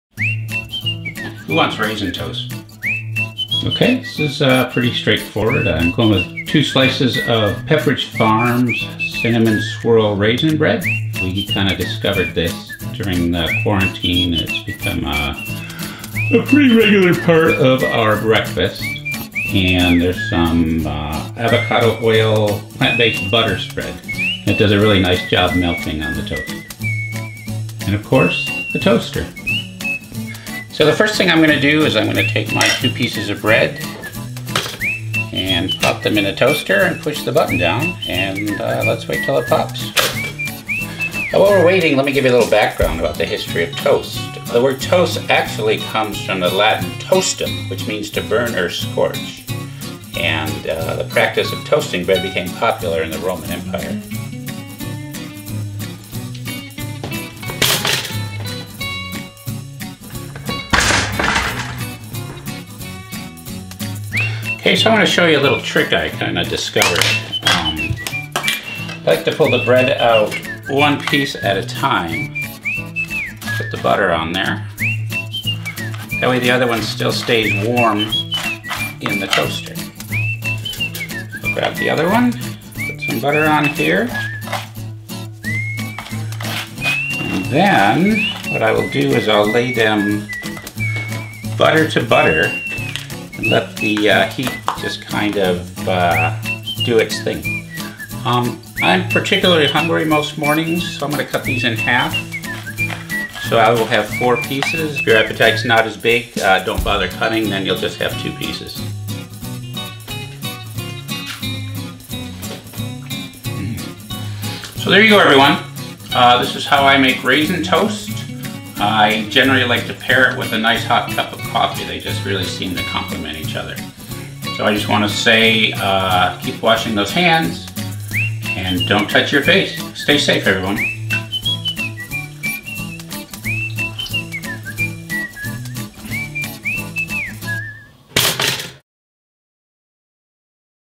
Michael's Making the Best Raisin Toast!
Watch and marvel as Toast Master Michael Pfaendtner toasts up a batch of Raisin Toast! You will be amazed!!!